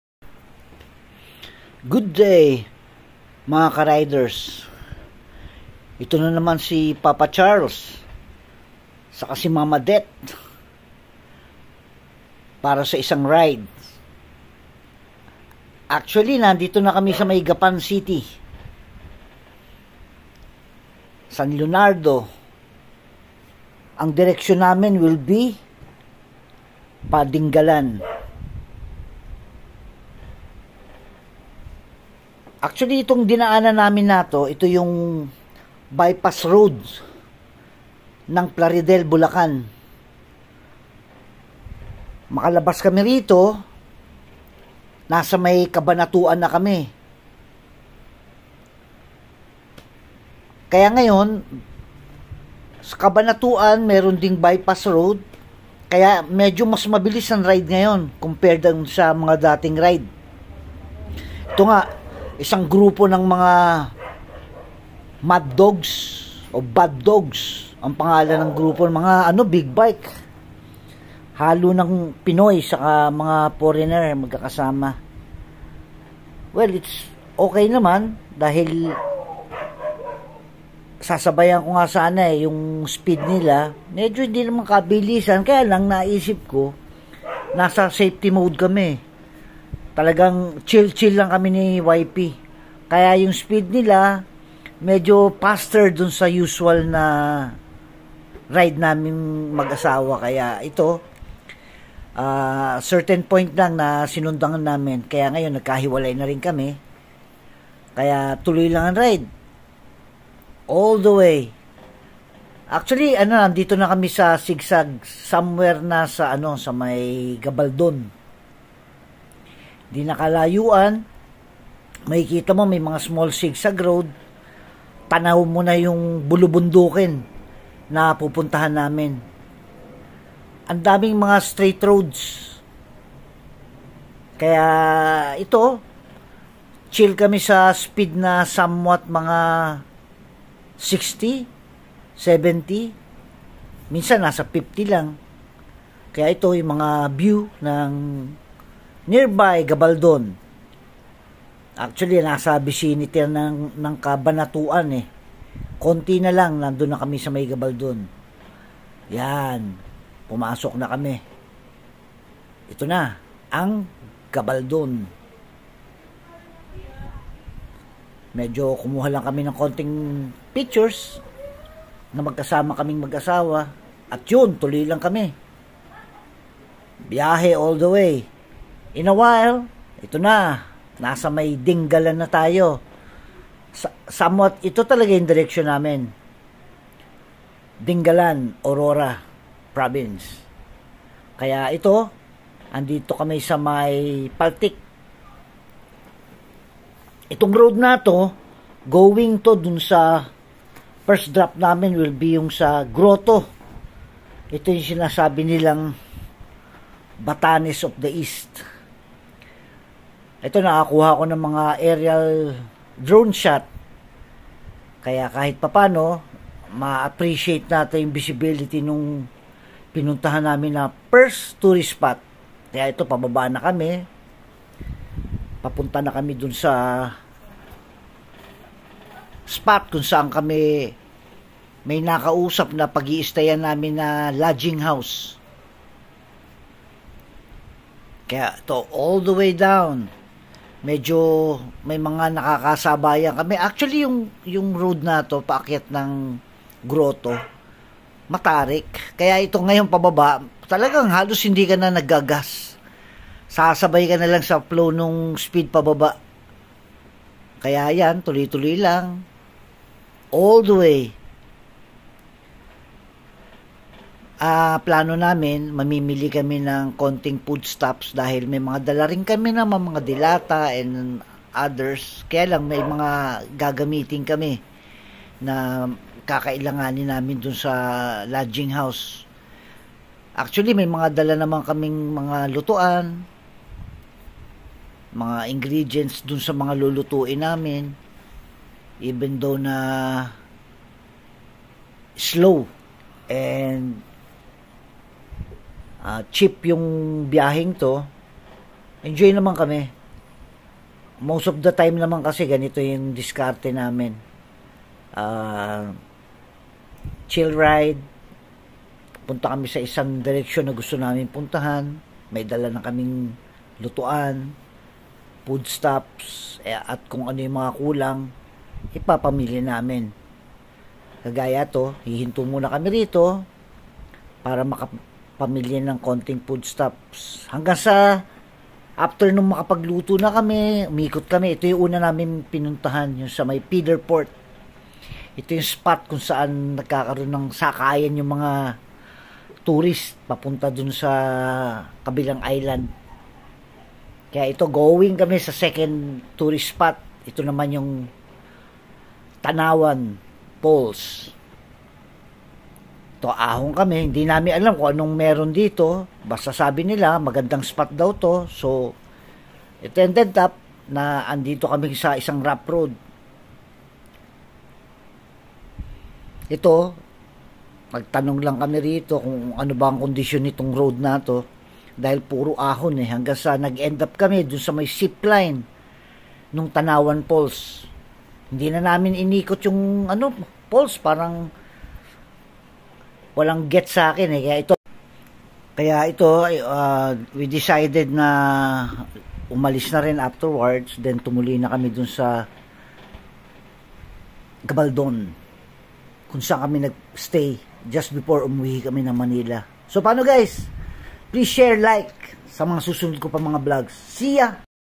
Dingalan Aurora Province Here we Come ...
This is our usual getaway with my wife. Our passion for travel and ride ..